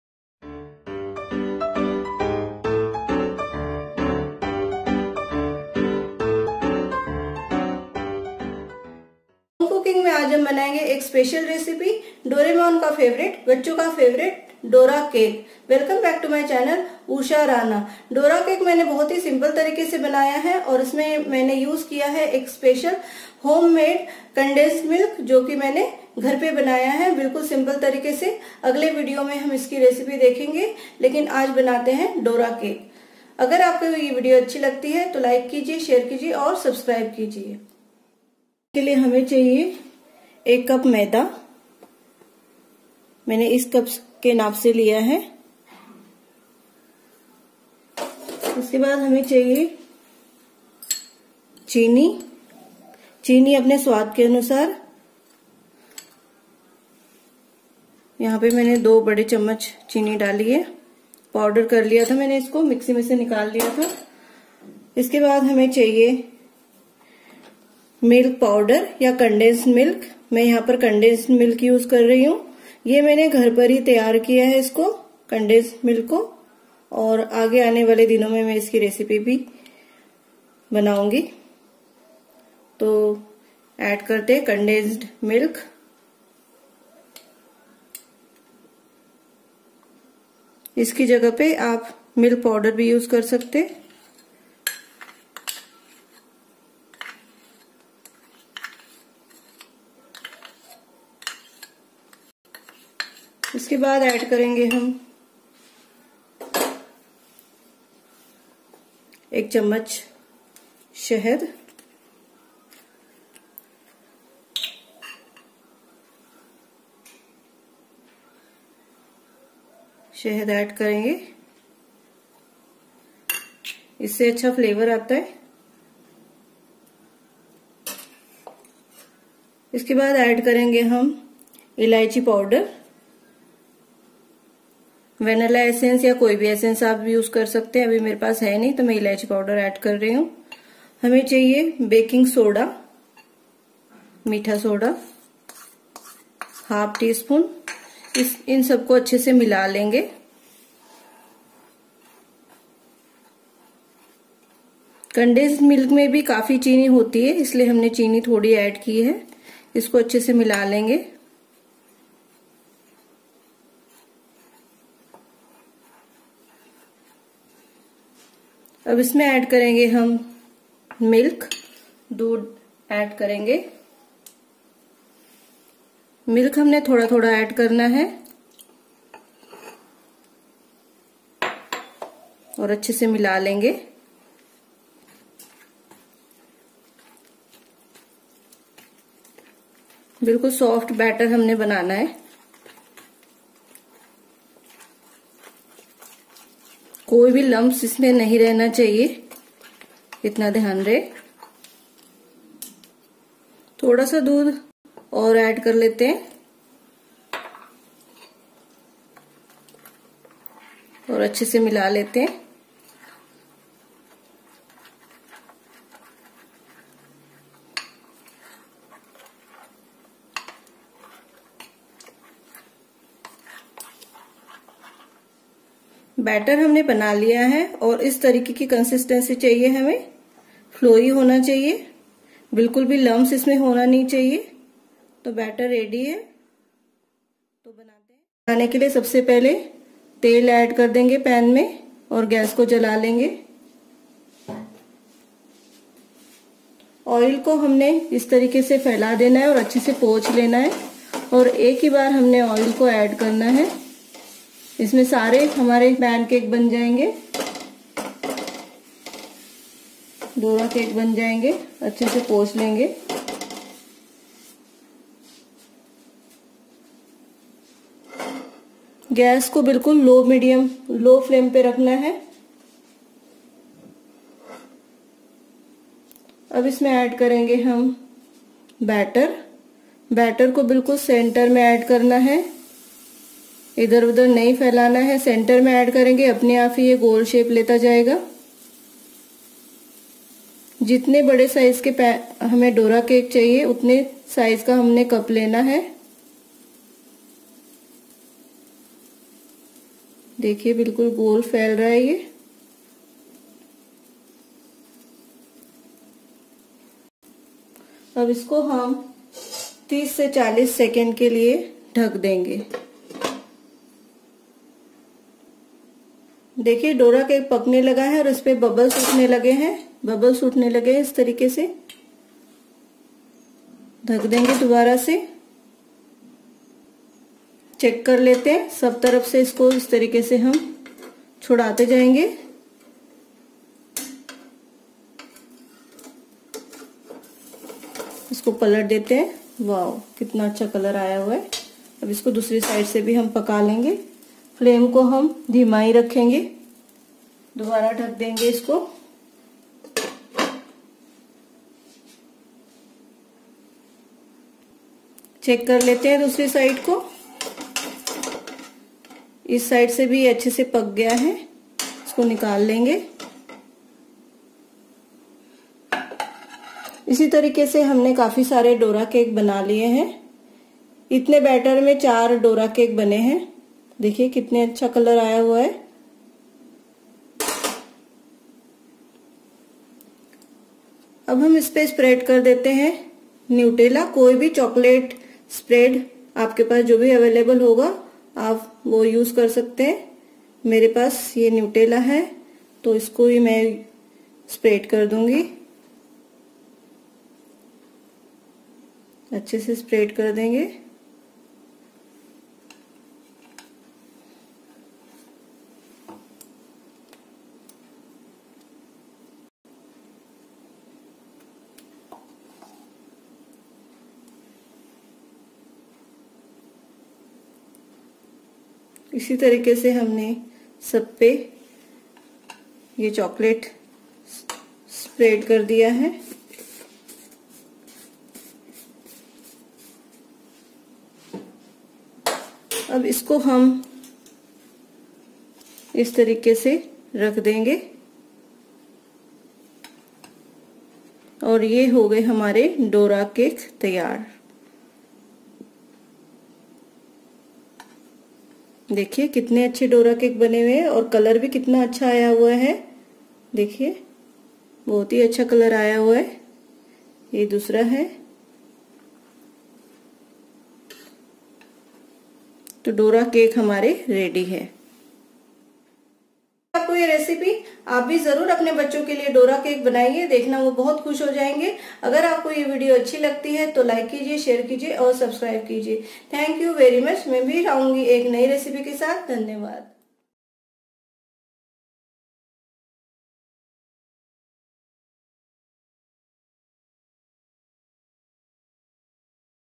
Dora Cake In 5 Minutes डोरा केक
Ingredients
Maida
Powdered sugar
Milk powder or condensed milk
Vanilla essence or elaichi powder
Baking soda(meetha soda)
Honey
Milk
Nutella or any chocolate spread